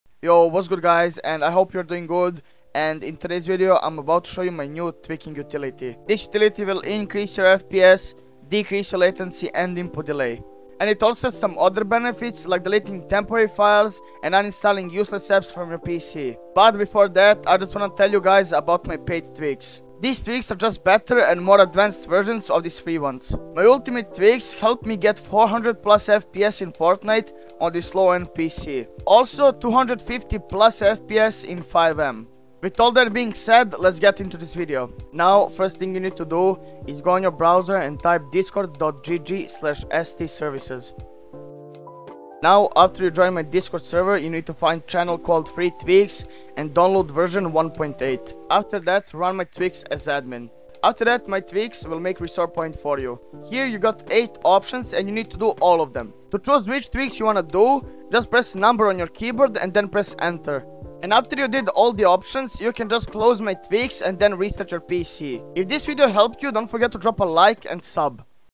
*ULTIMATE* Fps Boost Tool🔧(Boost Fps, Lower Input Delay & Latency) 400 FPS ON GT730
16 yold pc tweaker 170+ satisfied customers

PC SPEC
GT 730 ZOTAC 4GB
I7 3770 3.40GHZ
16GB DDR3 RAM 1333 MHZ
250 GB SSD
500GB HDD
MOTHERBOARD SOME DELL IDK 10+ YEARS OLD
FPS IN FORTNITE 144 LOCK
FPS IN VALORANT 180+
FPS IN FIVEM AT THE RAMPS 230-270